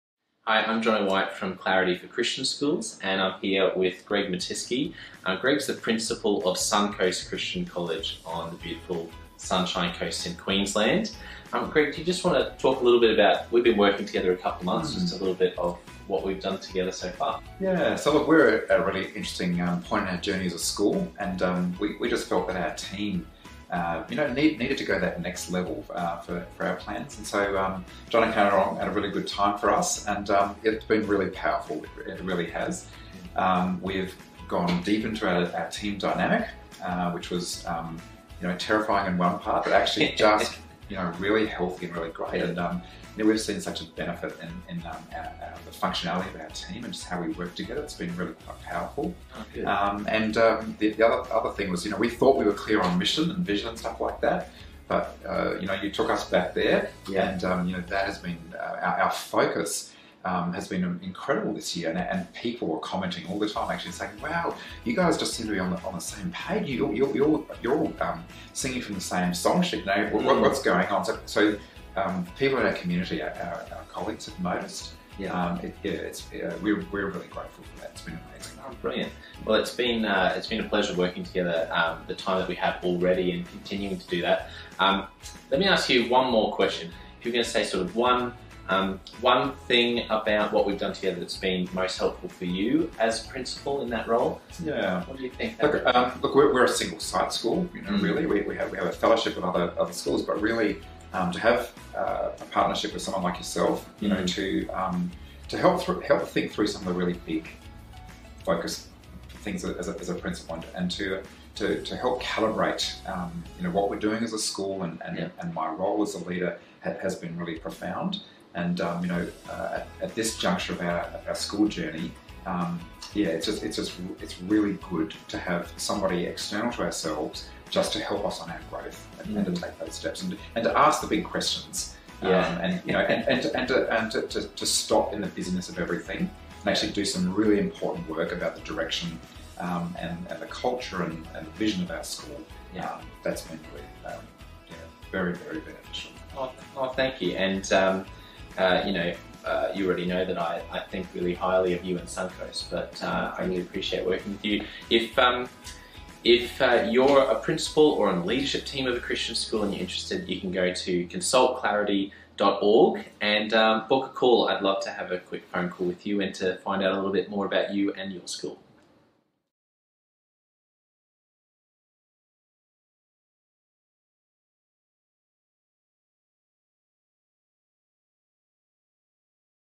Clarity testimonial by Greg Mattiske from Suncoast Christian College
Greg Mattiske, principal of Suncoast Christian College talks about how his school has been working with Clarity for Christian Schools and what impact that work has had across the school in a short time.